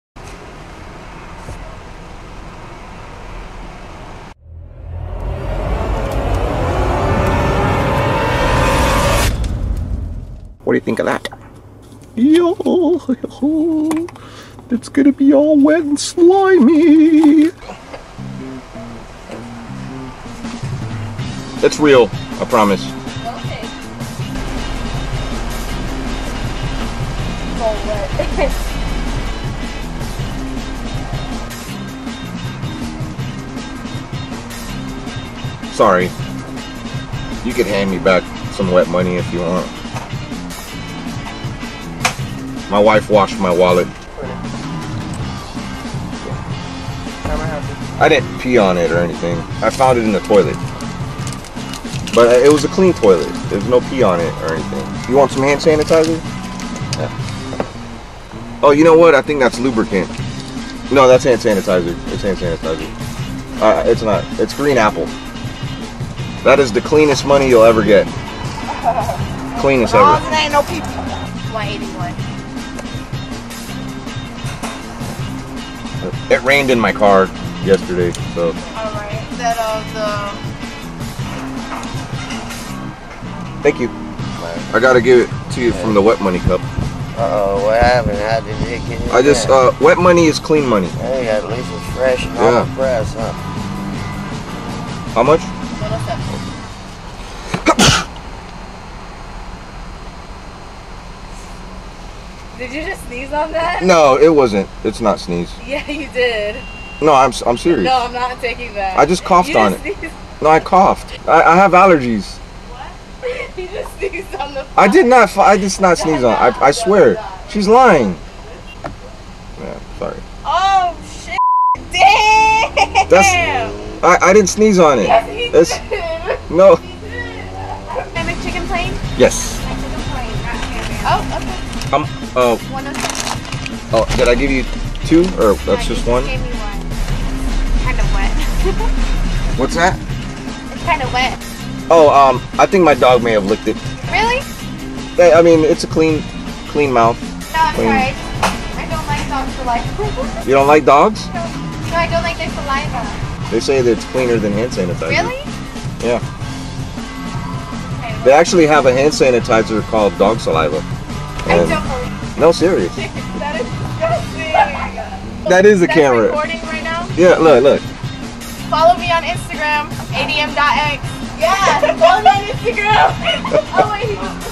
PAYING WITH WET MONEY AT THE DRIVE THRU
Today Omargoshtv decided to go to the drive thru todo a funny prank and pay the workers with soaking wet money. This harmless and funniest prank got lots of laughs from the drive thru workers when omargoshtv greeted them and beats a gold digger prank anyday.